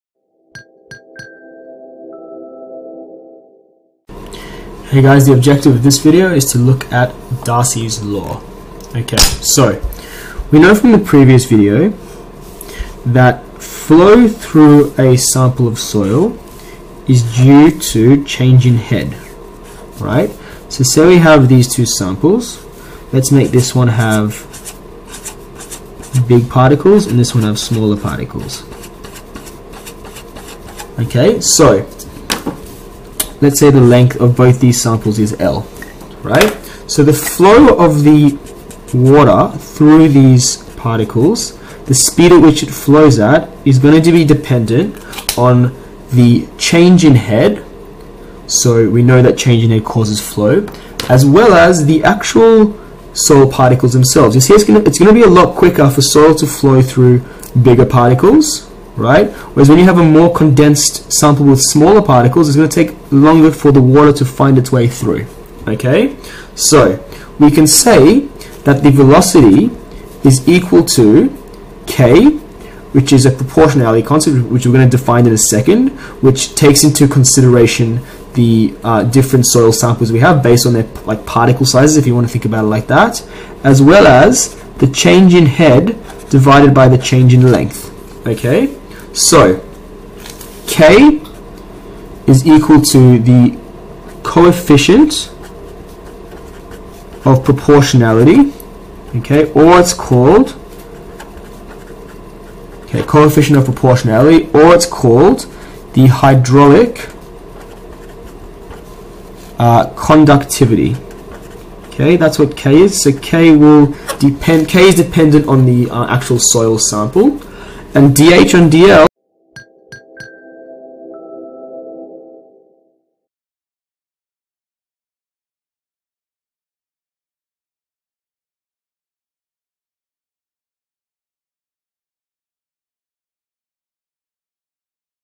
Introduction to Darcy’s Law | Soil Mechanics
For more FREE video tutorials covering Head and Water Flow through Soils and Soil Mechanics.

In this video we examine Darcy’s Law and its relationship with water flow rate and velocity, taken as a function of the hydraulic gradient the coefficient of proportionality.

This video is part of the topic head and water flow through soils which examines the definition of head as the combination of elevation and pressure change. Darcy’s law is explained and given as a combination of the coefficient of proportionality and the hydraulic gradient.